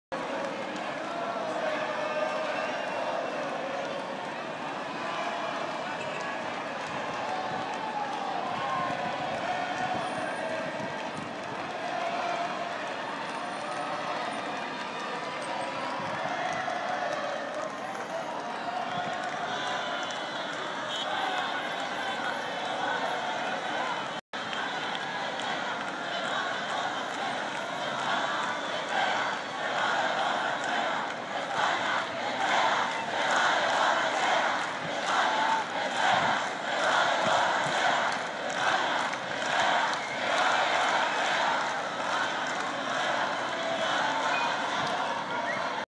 Spanish celebrate in Dublin's O Connell street
2010 winner of the world cup.. Spainish  celebrate on o Connell St Dublin .